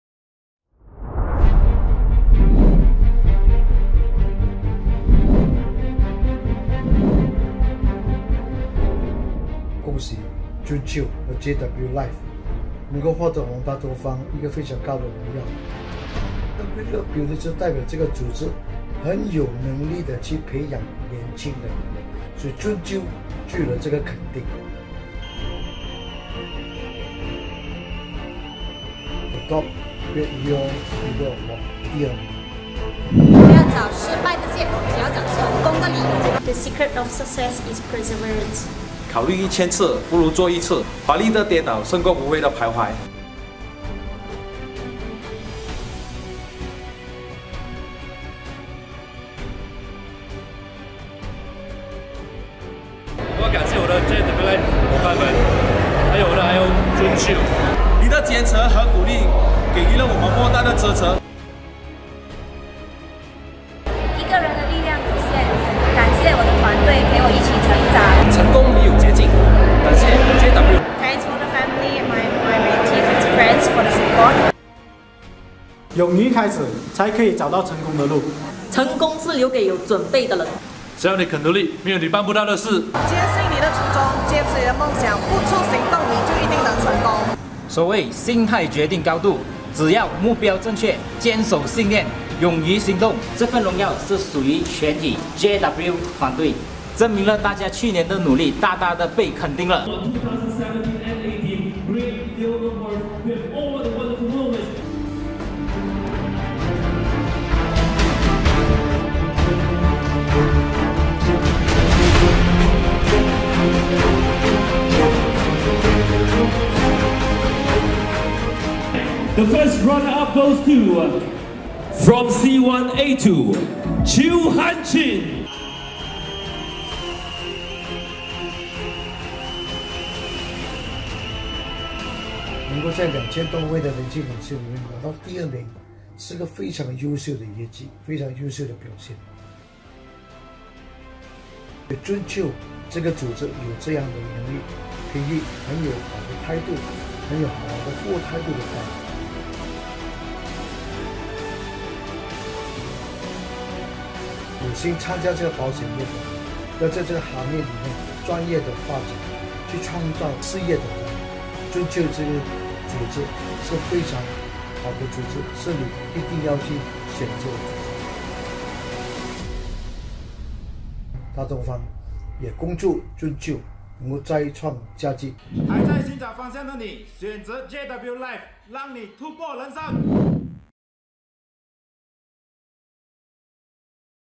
JWLife - GREAT LEO AWARDS ( Great Eastern )
A wonderfull night we celebrate success and joy together !!!
🏆🏆🏆Gongratulation to all JW Team qualifiers for your achiever🏆 🏆🏆
Great Eastern Life Top Leo Builder Award